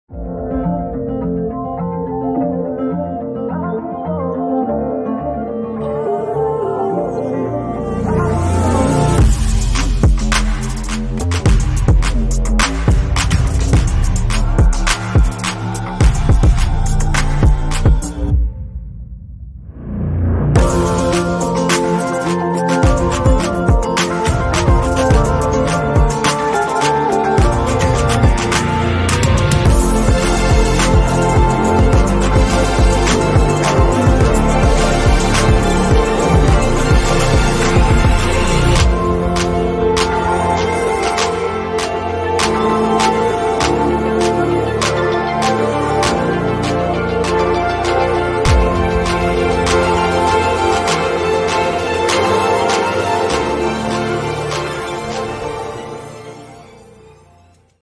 VTT enduro st Nazaire le désert " le Parquet"
VTT enduro dans la  Drôme à st Nazaire le désert sur la montagne" le Parquet"  un petit coin de paradis avec une vue imprenable et un single magnifique...